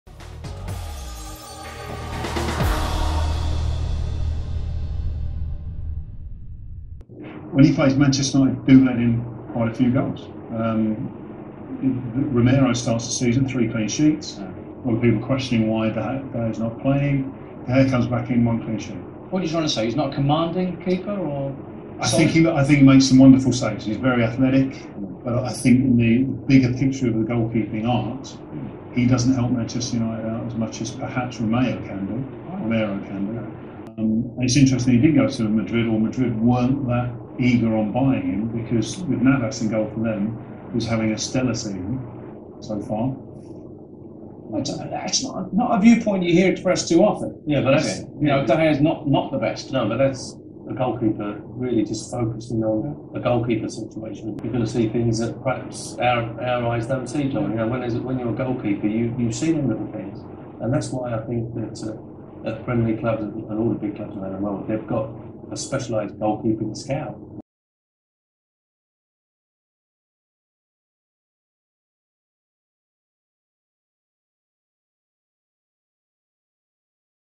David James Claims Sergio Romero Is Better Than David De Gea & That Real Madrid Didn't Want Him !
Big thanks to @ishmam7 for getting the footage for me

Thanks